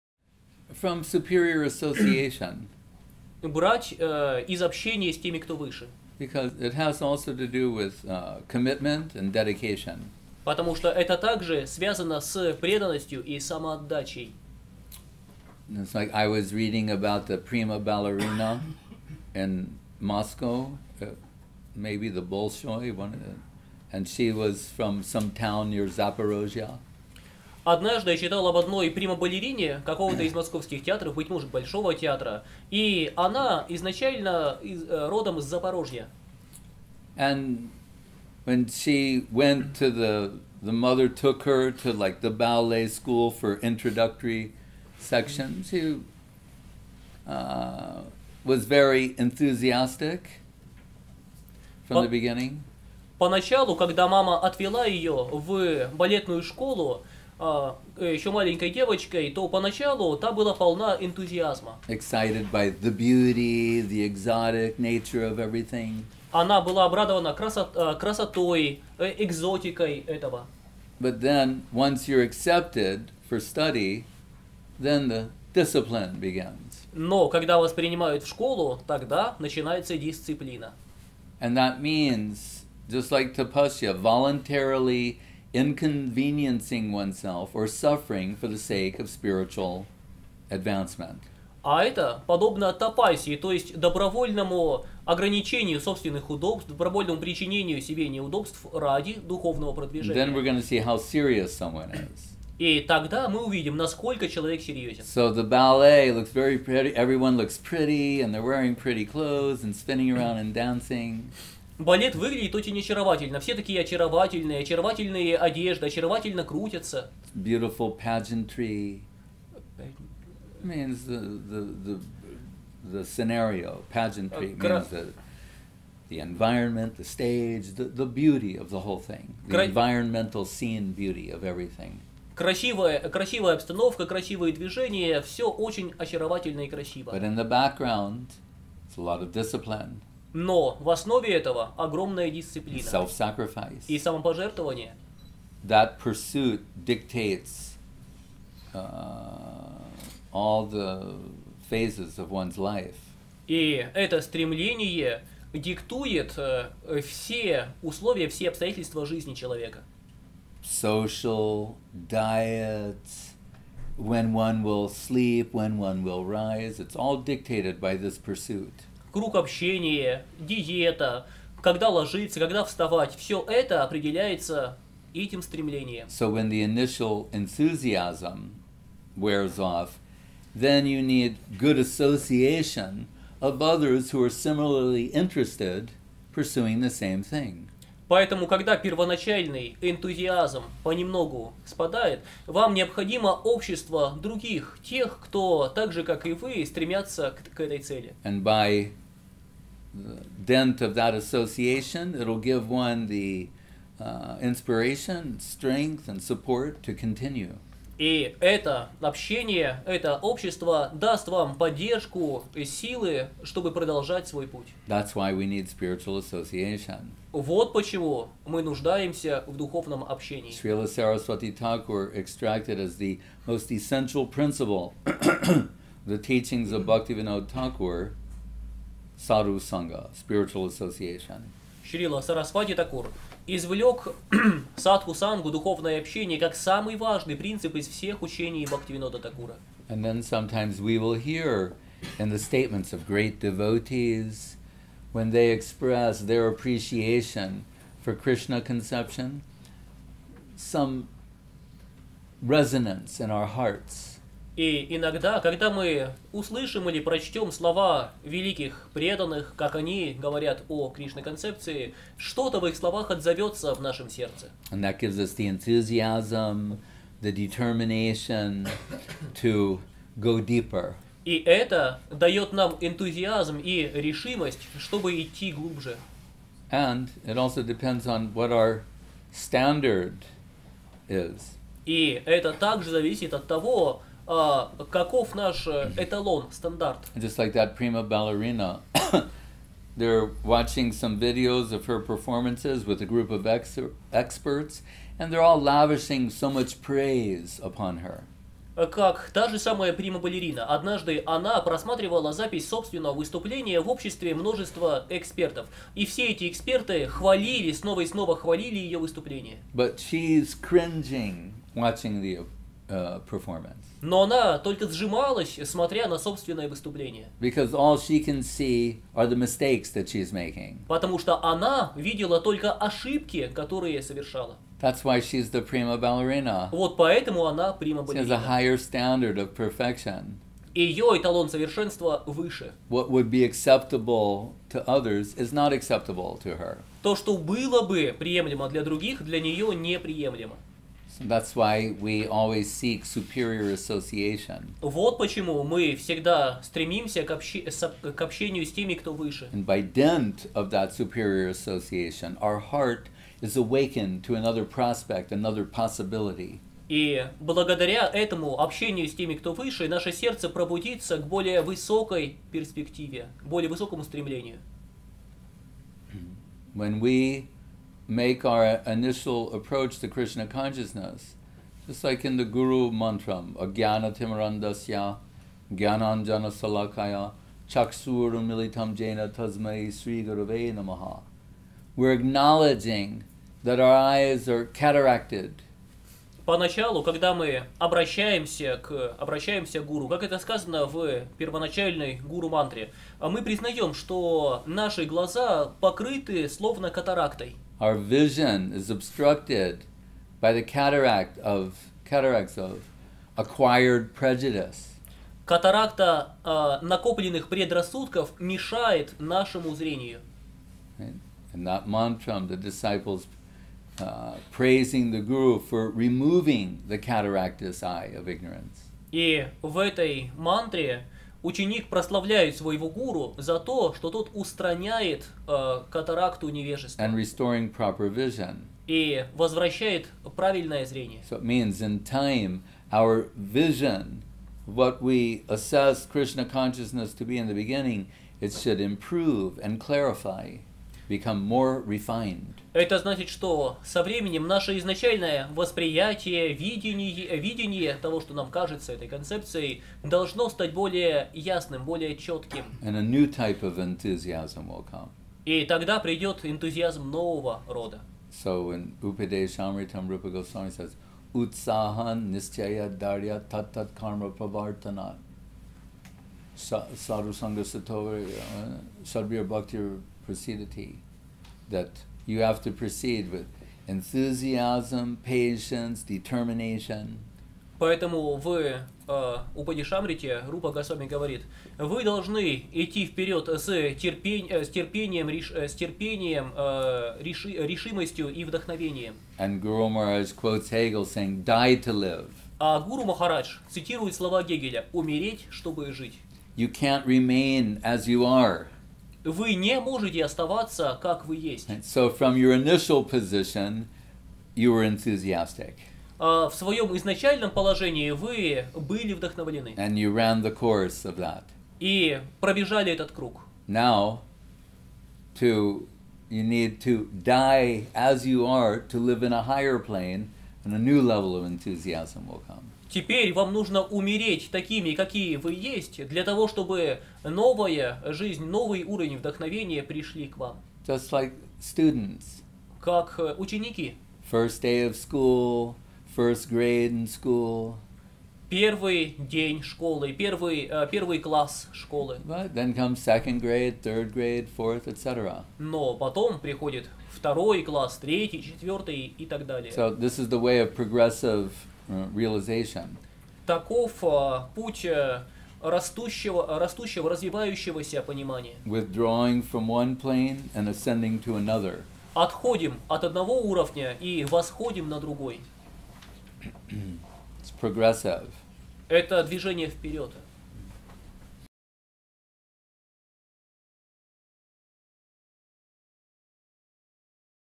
Энтузиазм. От чего зависит и где его черпать?
Киев, студия йоги "Vedalife", 16 января 2012 года. Шрила Б.С.Госвами Махарадж
00:05 - Энтузиазм нужно черпать от общения с теми, кто выше нас.
04:40 - Очень важно какой мы ставим себе идеал, стандарт.
06:30 - Наше видиние искажено, и помочь нам может только подлинный садху, святой.